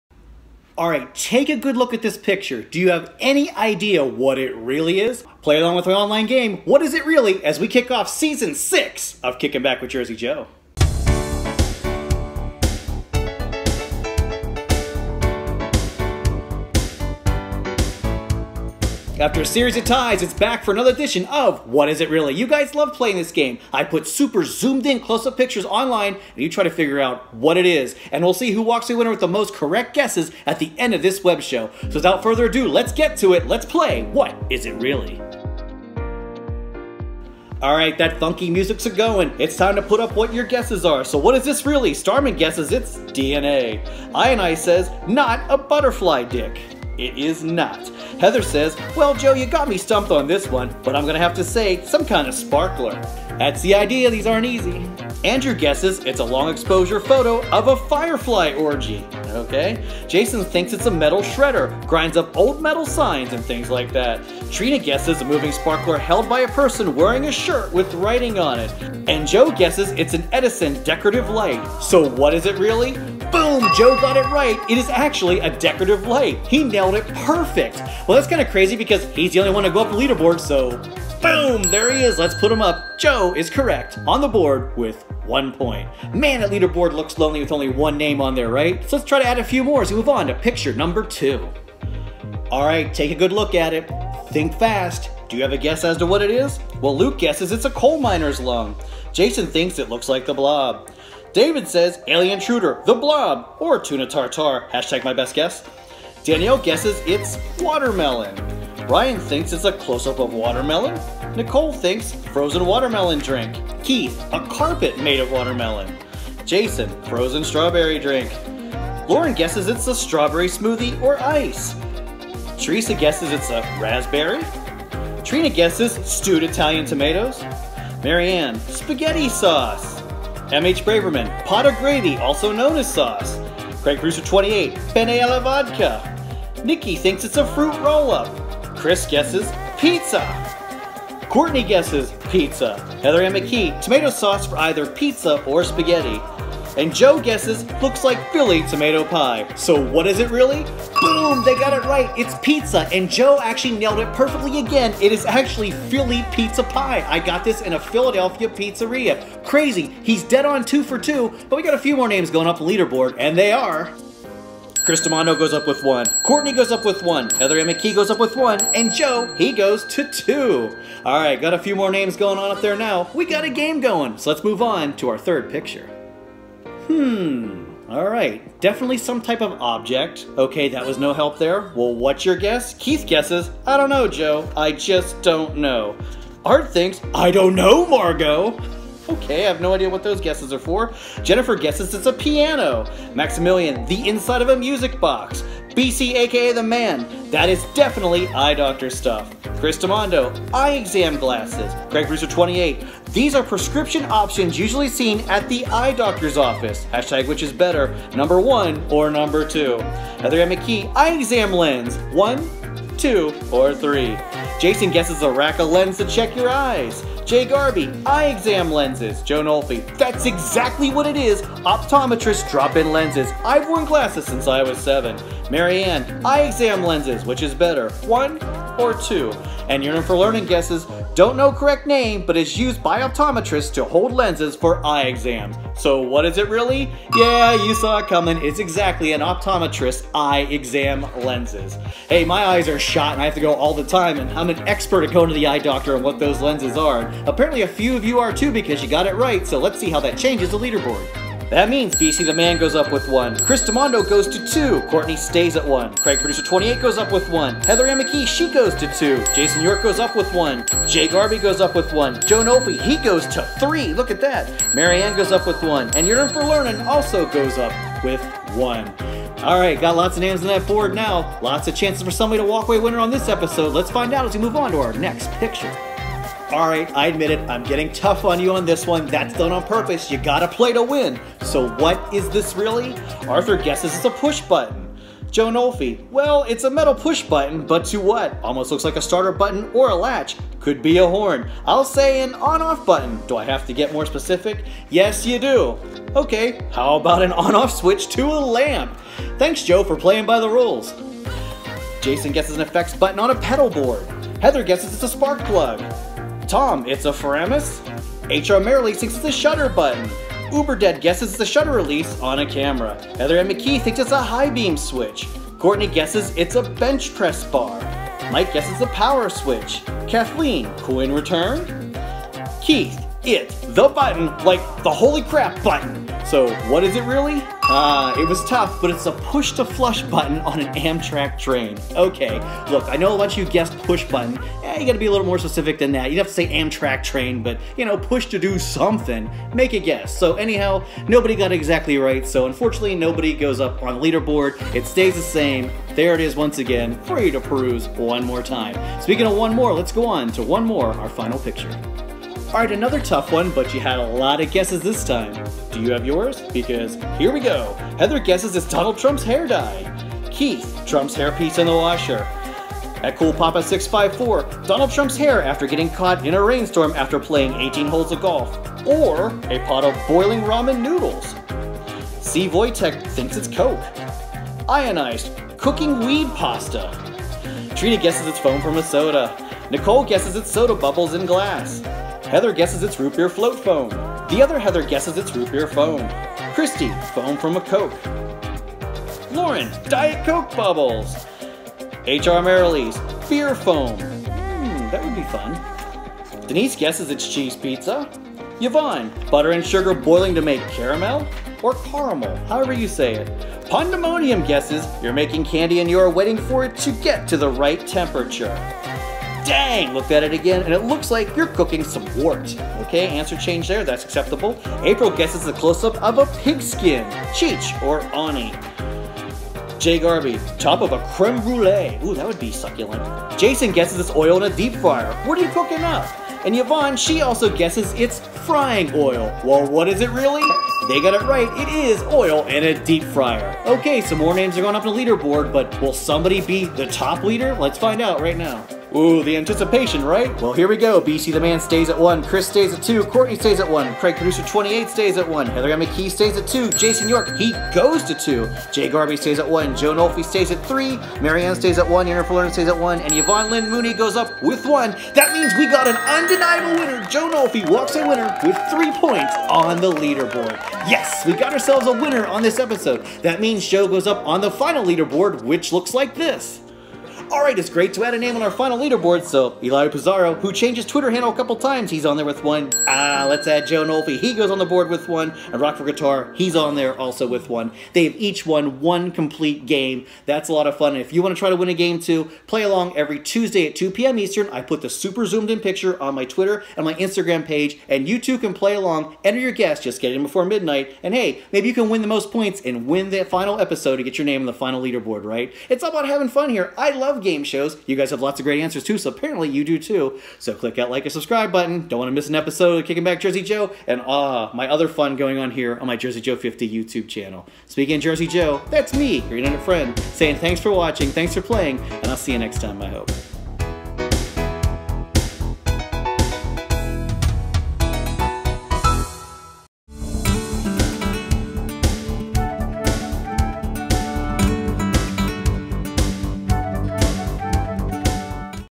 What is it Really? # 8 [Jersey Joe # 457]
Can you figure out what's really happening in these zoomed in photos?  Play along with the latest round of WHAT IS IT REALLY? and see if you can top the leader board!

KICKING BACK WITH JERSEY JOE # 457